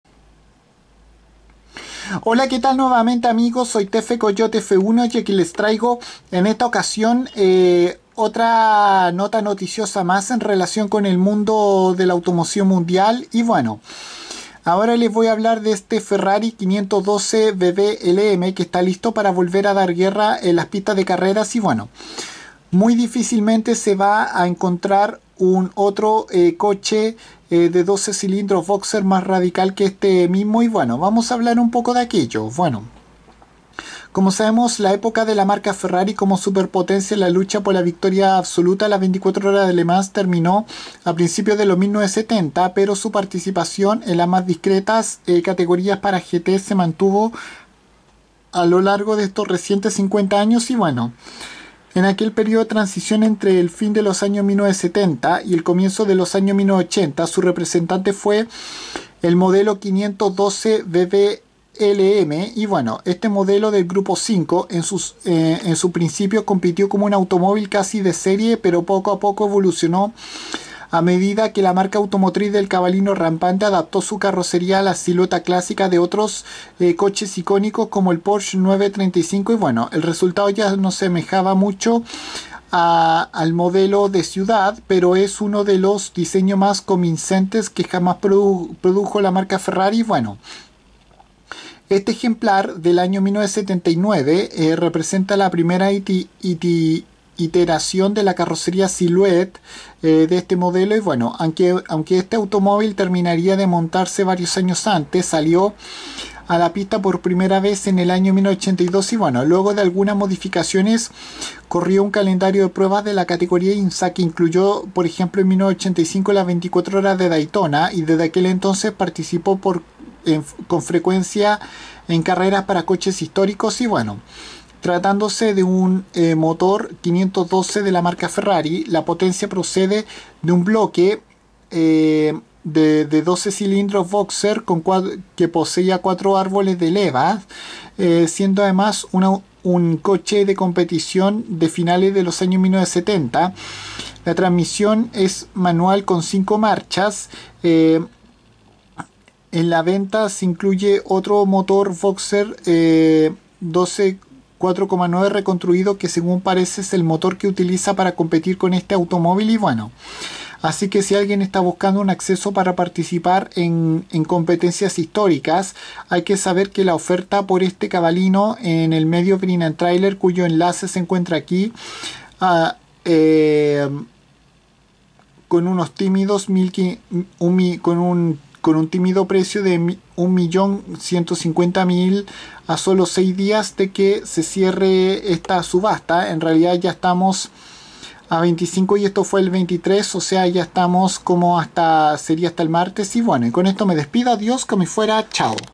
A la venta este Ferrari 512 BB LM
Este es un peculiar coche de competición de Maranello que posee un motor bóxer de 12 cilindros que ahora está en subasta